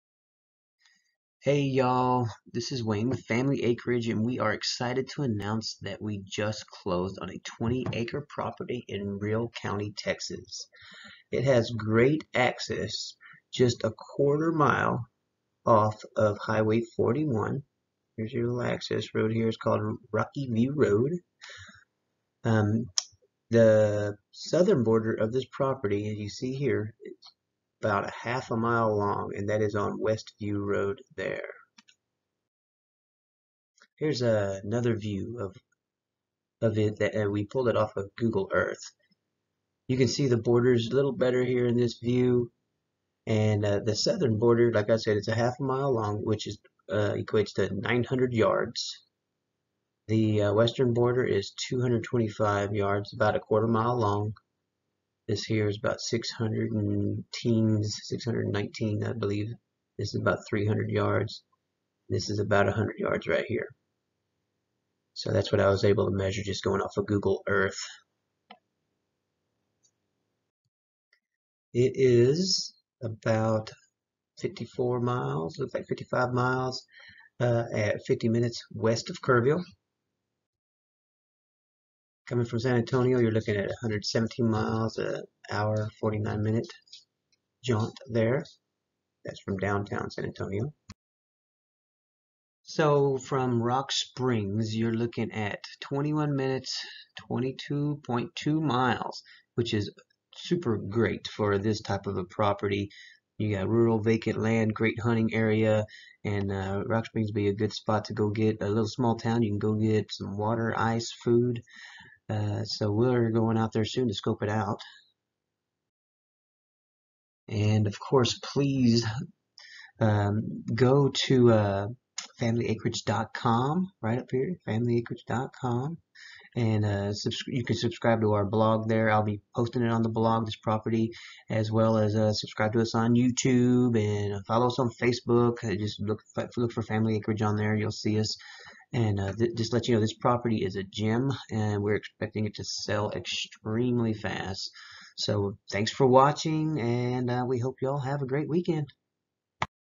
Land For Sale!!! - Coming Soon - We are SUPER Exited about these 20 Acres in Real County TX
Hey y’all! We are excited to announce that we just closed on a 20 Acre property in Real County Texas. It has great access just .25 mile off of TX-41. The Southern Boarder of this property runs ½ mile along West View Road.
The second picture is another view of the property that we pulled off of Google Earth. You can see the borders better in this view. The southern border is 900 yards long, the western board is 225 yards long, and the Eastern boarder is 100 yards long.
55 miles/50 Min West of Kerrville Tx
1 hr 49 min or 117 miles from San Antonio
Only 22 miles or 21 min from Rock Springs Tx, which will be your closest place for food, water and Ice.